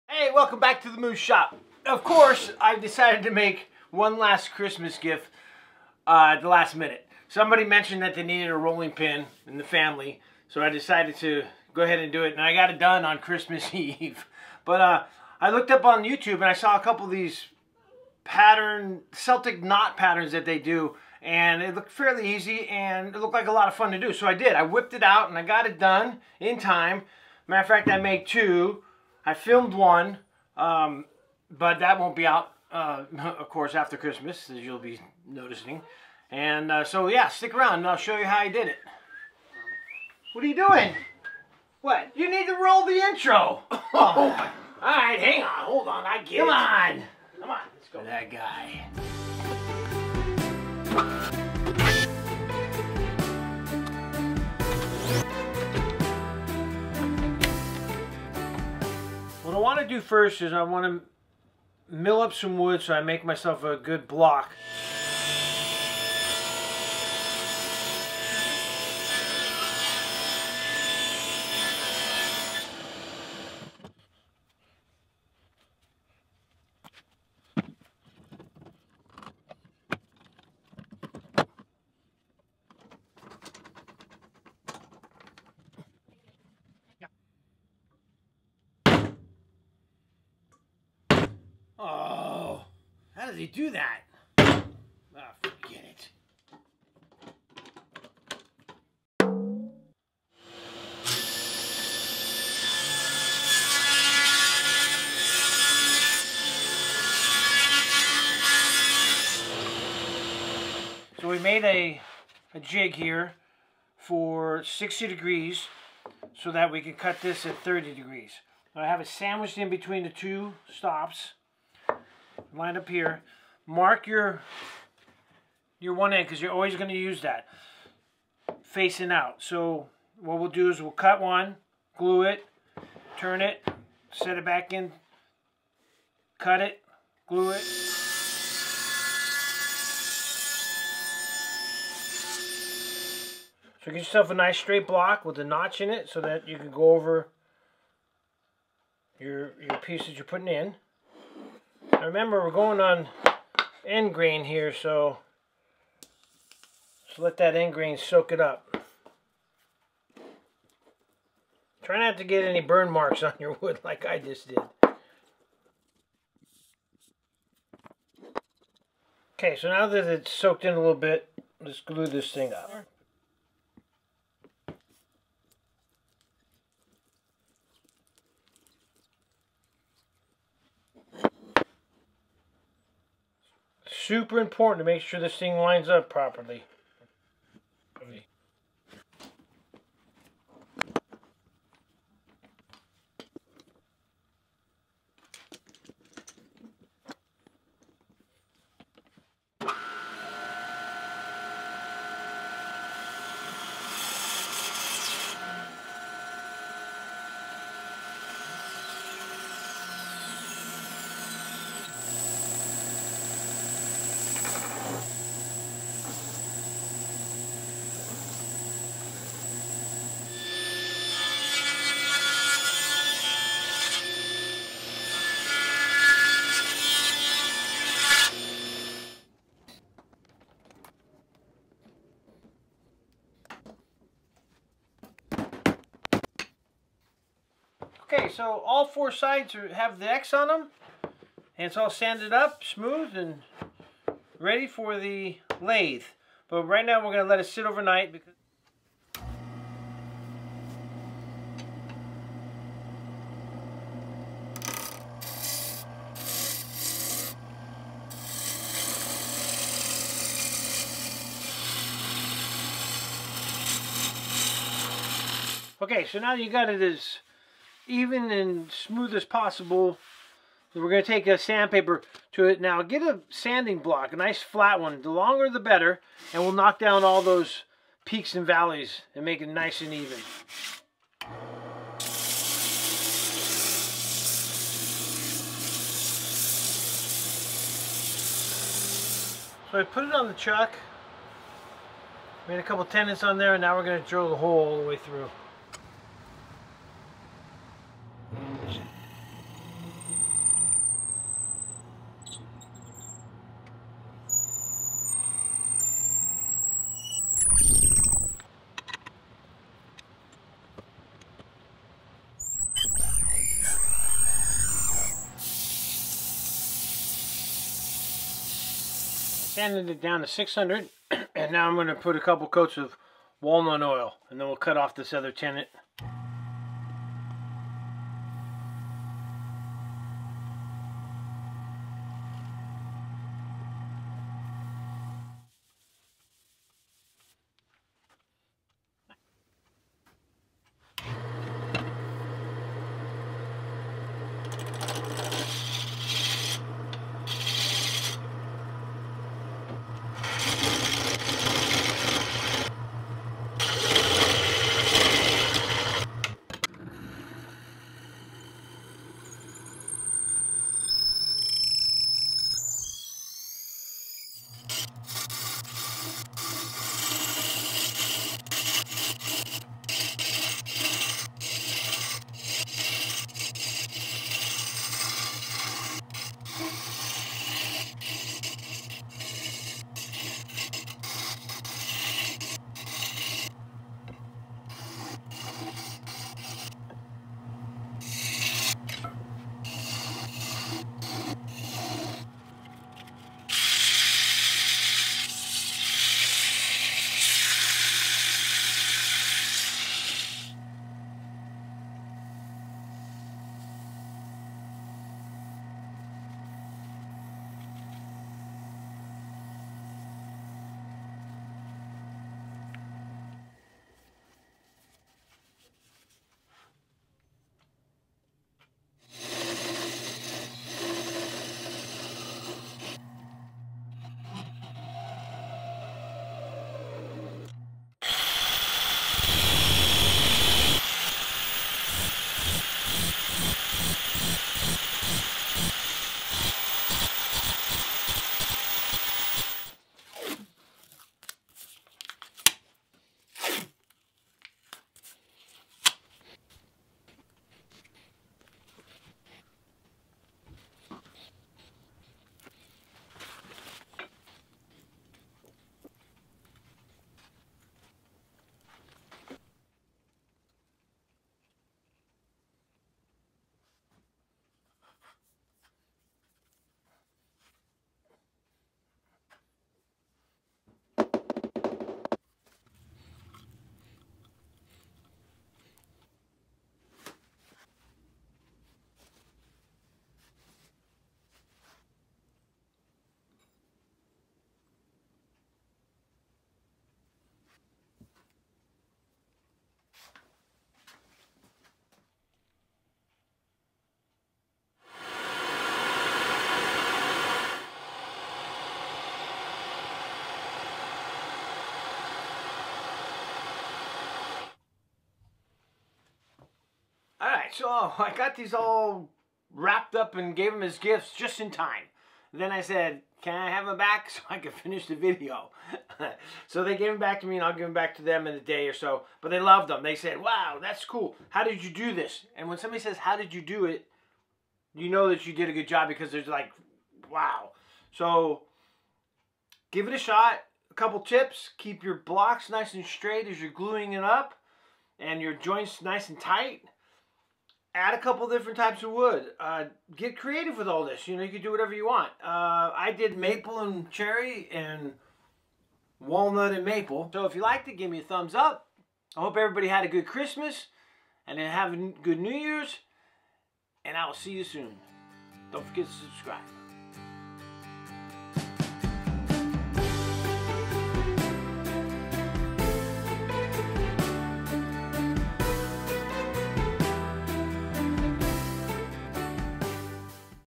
Super Cool Rolling Pins!! Easy to Make
I needed one lasted gift for Christmas, with only a few days left. So I decided to make a couple of Rolling Pins. One of them would go to a family member who mentioned they needed a Rolling Pin a few weeks ago, the other to Mom. I did a little research and came across this Celtic knot pattern. Which looked really cool and not too hard to do, so I when for it. I picking out Maple, Walnut and Cherry wood to mix and match on the two pins. The first Rolling Pin I used Maple with Cherry handles and a Cherry Celtic Knot. The other one I used Walnut with Walnut handles and a Maple Celtic knot. They both turned out really nice.I'm not sure which pin I like better. Let me know which one you like in the comments below. Overall it's a pretty easy build. Give it a go.

Any questions

Items used:

Metal rods

5 minute epoxy

Wood glue

Tape measure

Jet clamps

Easy Wood turning tools

Jet lathe

PVC Pressurized water system for a truck